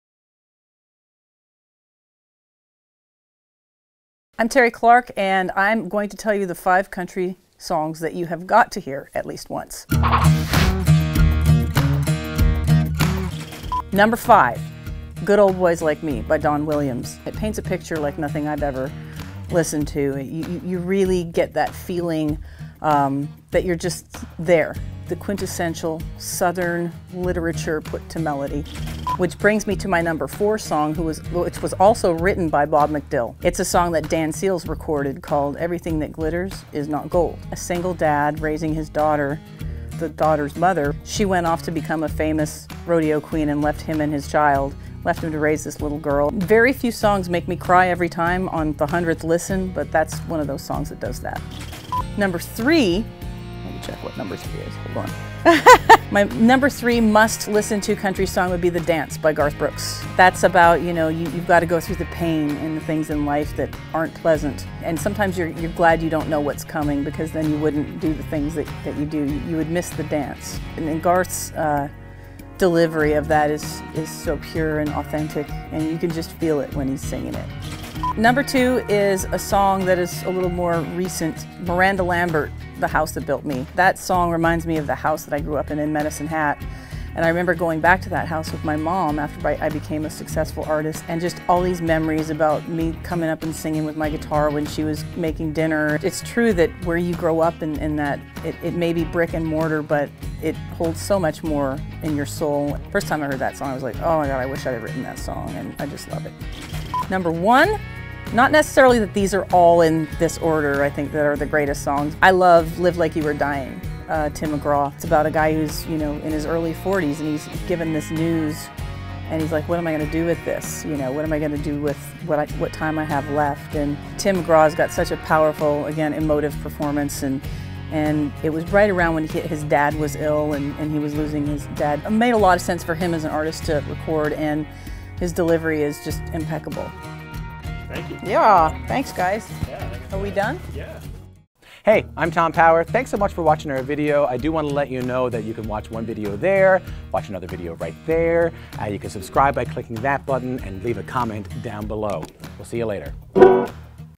Terri Clark’s 5 country songs you need to hear at least once
Canadian country pioneer Terri Clark shares her five essential country songs.
Camera: David Mewa
Editing: Dennis Chan
Producer: Jesse Kinos-Goodin